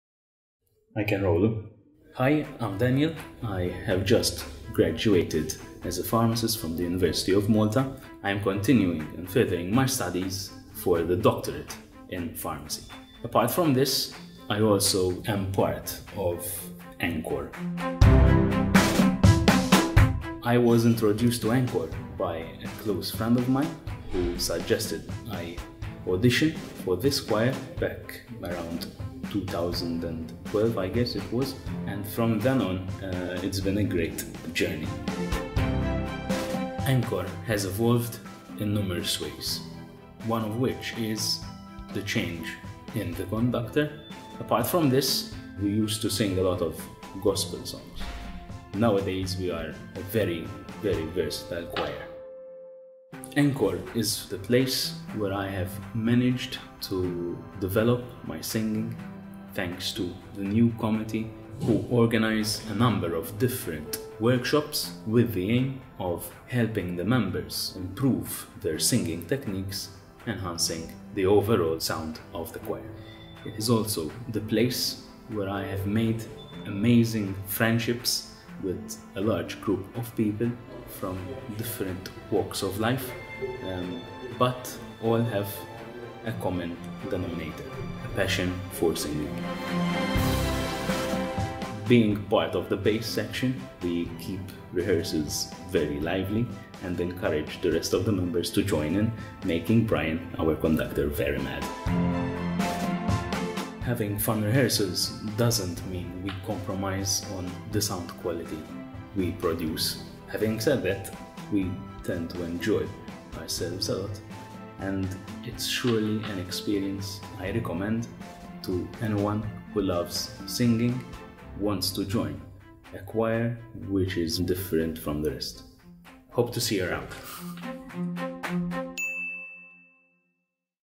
Meet teamorange Ep.6 // Daniel
Time for a bubbly bass to take centre stage! Meet Daniel -merrymaker and joker extraordinaire - learn a little more about him, and find out what he likes best about EnKor.